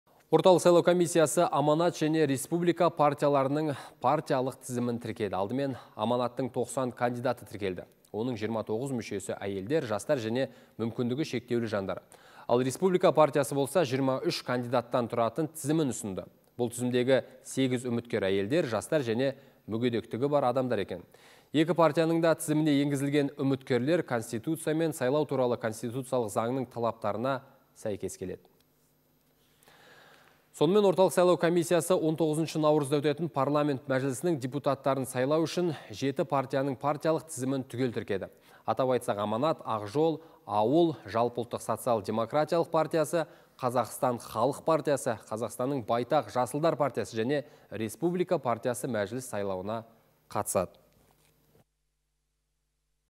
Орталық сайлау комиссиясы «Аманат» және «Республика» партияларының партиялық тізімін тіркеді
Орталық сайлау комиссиясы «Аманат» және «Республика» партияларының партиялық тізімін тіркеді. Алдымен «Аманаттың» 90 кандидаты тіркелді. Оның 29 мүшесі әйелдер, жастар және мүмкіндігі шектеулі жандар. Ал, «Республика» партиясы болса 23 кандидаттан тұратын тізімін ұсынды. Бұл тізімдегі 8 үміткер әйелдер, жастар және мүгедектігі бар адамдар екен. Екі партияның да тізіміне енгізілген үміткерлер конституция мен «сайлау туралы» конституциялық заңның талаптарына сәйкес келеді.
Сонымен, орталық сайлау комиссиясы 19 наурызда өтетін Парламент мәжілісінің депутаттарын сайлау үшін жеті партияның партиялық тізімін түгел тіркеді. Атап айтсақ, «Аманат», «Ақ жол», «Ауыл», Жалпыұлттық социал-демократиялық партиясы, Қазақстан халық партиясы, Қазақстанның «Байтақ» жасылдар партиясы және «Республика» партиясы мәжіліс сайлауына қатысады.